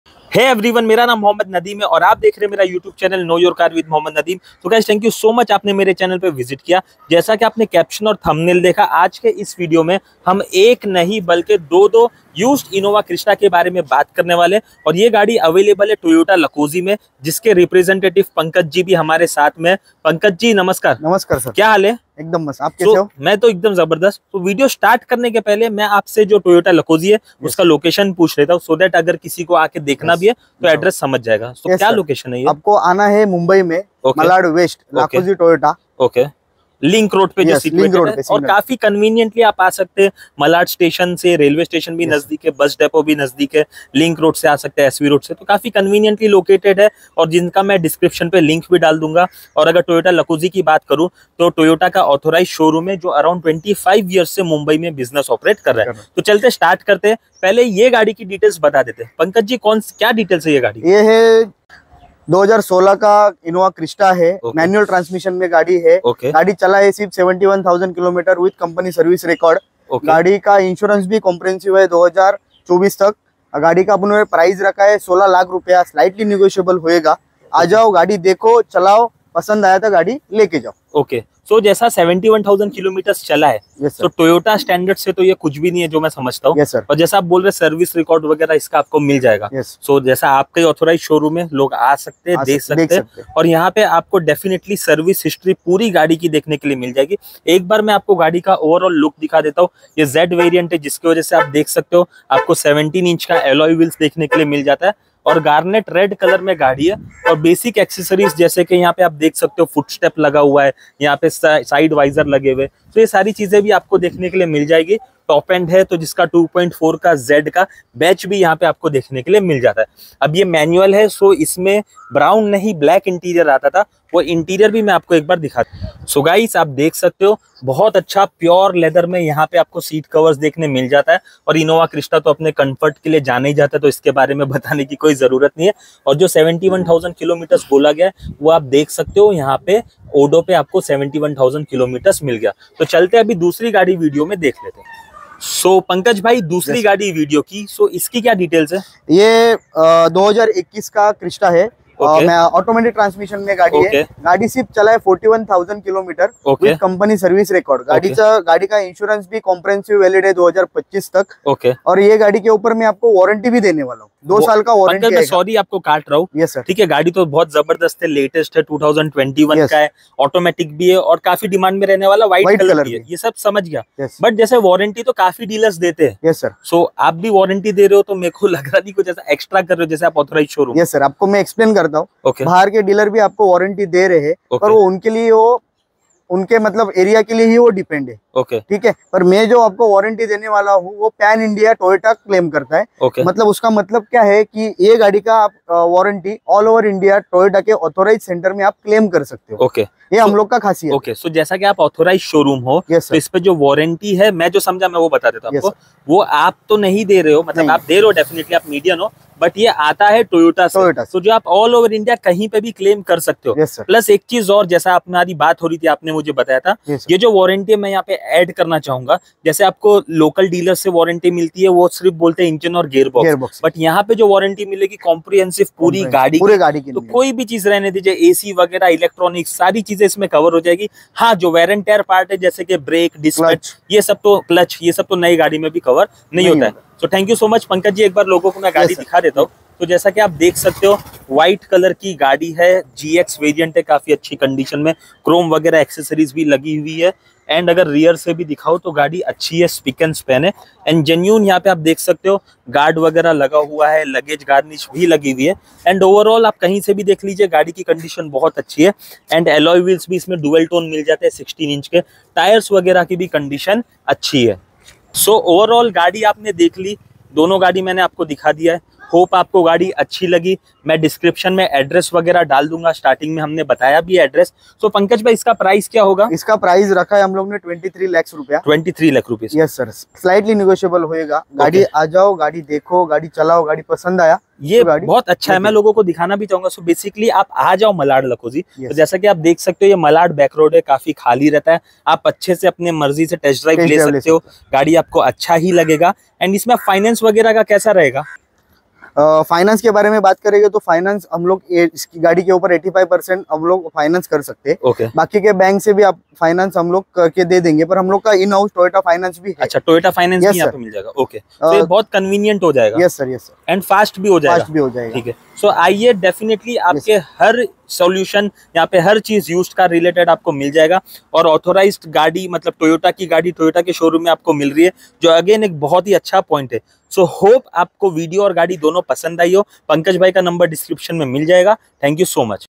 Used Car Toyota Innova Crysta Auto & Manual for Sale in Mumbai| Call Pankaj Patil📲 7304186429
Hey everyone,
In this video you can see a good condition Toyota Fortuner 4X4 for Sale In Mumbai and its reviewas per below details:

1. Model : Toyota Innova Crysta GX Auto and ZX MT for Sale
2. Owner Single
3. KMs: 42000 & 71000
4. City: Mumbai/Thane/ Maharashtra
6. INR: 1600000/2300000
7. Insurance Valid

Location: Mumbai Thane Maharastra